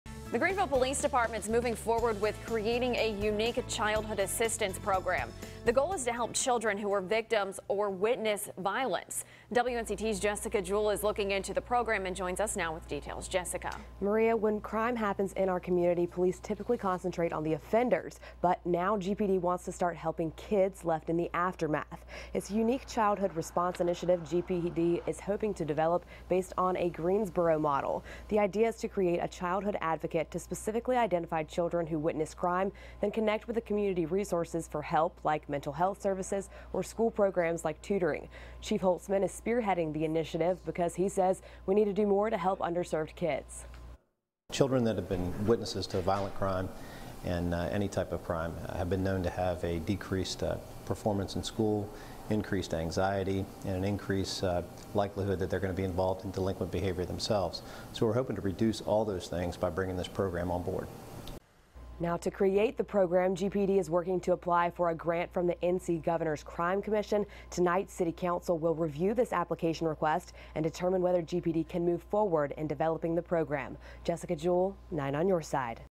GPD developing program to help children who witness violence
The Greenville Police Department is developing an innovative program helping children left in the aftermath of violence.